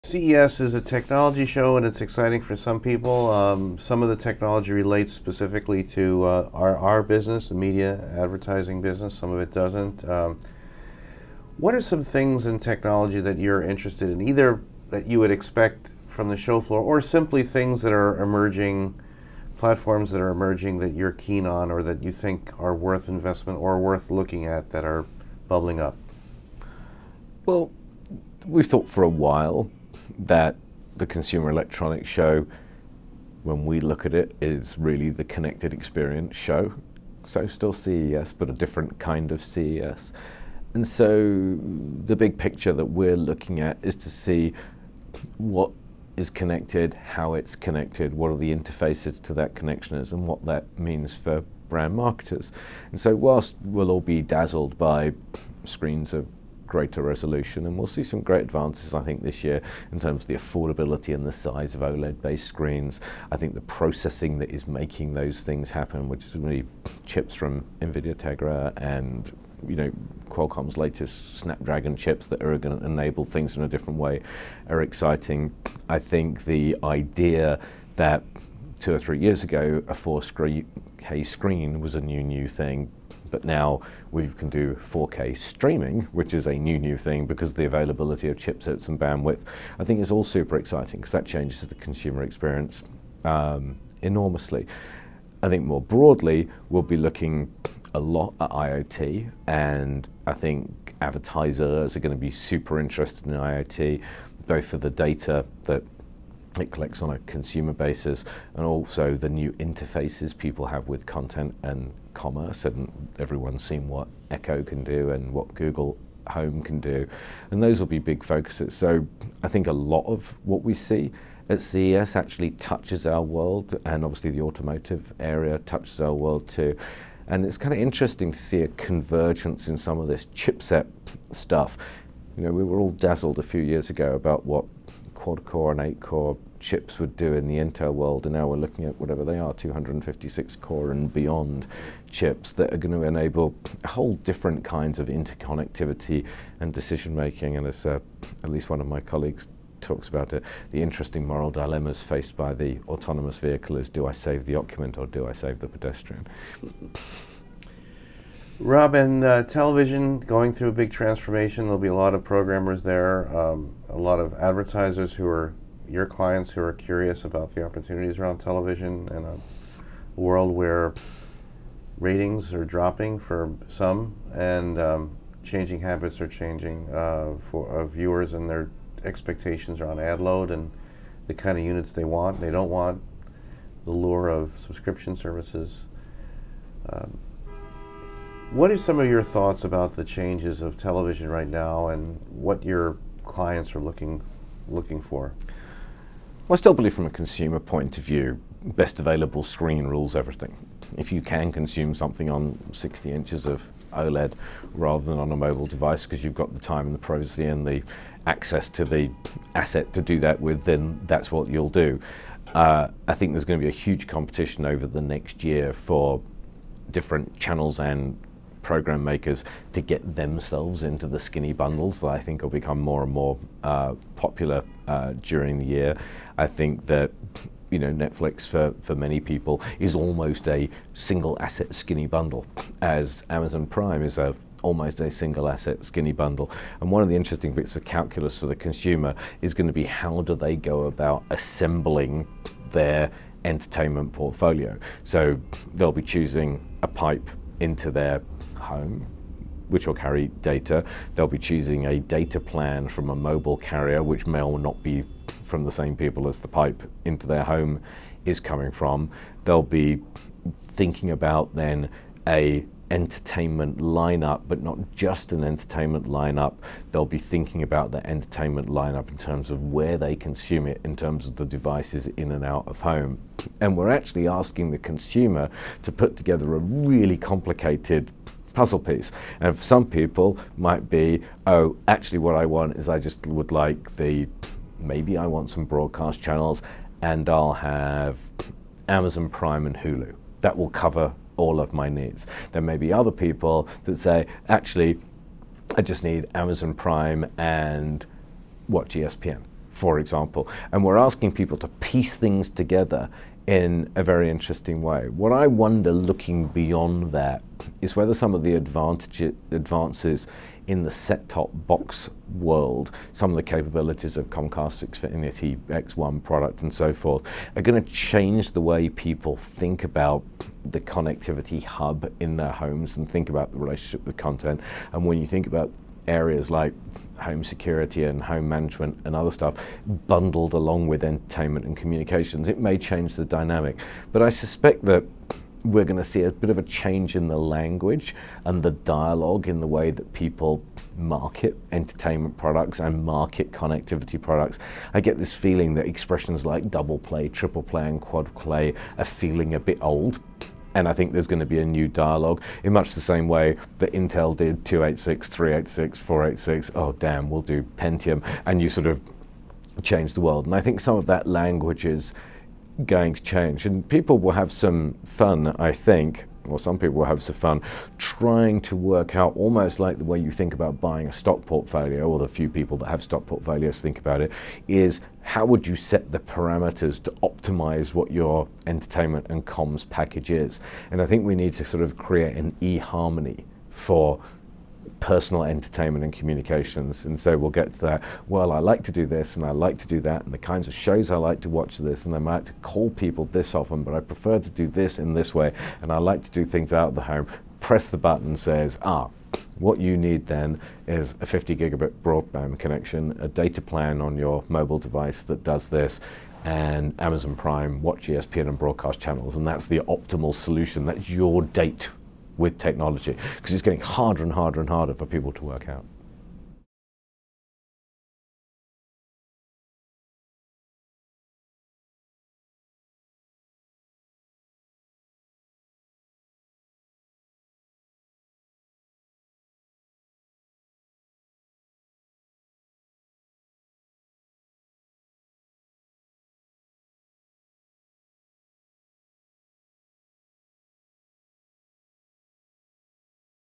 GroupM's Norman: Consumer Entertainment Choices A 'Complicated Puzzle'
One expects to see faster chipsets from the NVIDIAs and Qualcomms of the world at CES 2017, plus more dazzling television screens. But for TV programmers, a lot of the action will focus on skinny bundles and trying to figure out how consumers will figure out their individual relationships with entertainment content.

This is the prognosis from GroupM Chief Digital Officer Rob Norman, who thinks advertisers will be “super interested” in the Internet of things for the data that it collects and the new interfaces people have with content and commerce.

“Everyone’s seen what Echo can do and what Google Home can do and those will be big focuses,” Norman says in an interview with Beet.tv.

He sees “huge competition” in the coming year is how different channels and program creators will incorporate themselves into skinny bundles. For many people, Netflix and Amazon Prime can almost be considered single-asset skinny bundles.

“One of the interesting bits of calculus for the consumer is going to be how do they go about assembling their entertainment portfolio,” says Norman, citing a data pipeline to the home, a mobile data plan and perhaps bundled home security and management services.

“We’re actually asking the consumer to put together a really complicated puzzle,” Norman adds.

This is because of the plethora of choices for watching traditional broadcast channels and streaming video. “They’ll be thinking about their entertainment lineup in terms of where they consume it, in terms of the devices in and out of the home,” Norman says.

He suspects there might need to be changes in the way that entertainment products are marketed.

“I get this feeling that expressions like Double Play, Triple Play and Quad Play are feeling a bit old and I think there’s going to be a new dialogue,” says Norman.

Likening consumer choices to those involved in putting together a portfolio of stocks, Norman has a whimsical suggestion for making things simpler.

“I think we need to create a sort of eHarmony for personal entertainment and communications,” he explains. “It’s your date with technology because it’s getting harder and harder and harder for people to work out.”

This interview is part of our series “The Road to CES,” a lead-up series in advance of CES 2017. The series is presented by FreeWheel. Please find more videos from the series here.